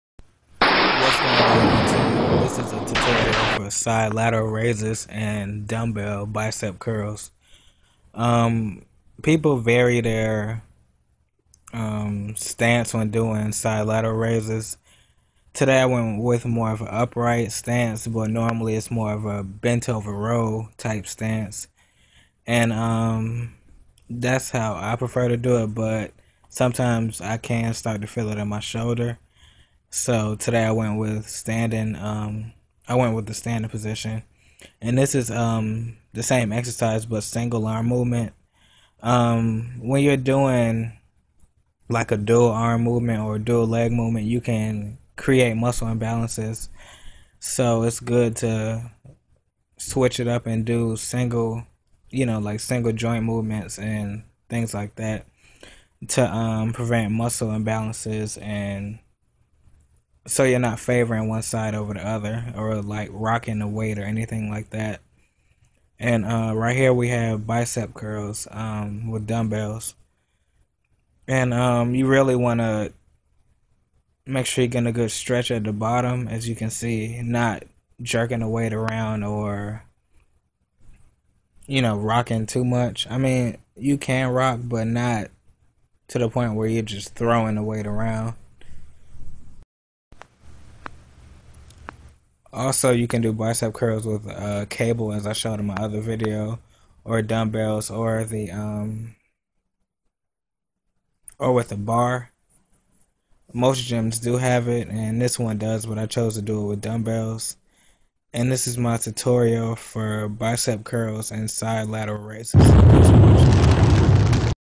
Proper form for side lateral raises & bicep curls
proper form for gaining size in your delts & biceps especially if you're looking for that bicep peak.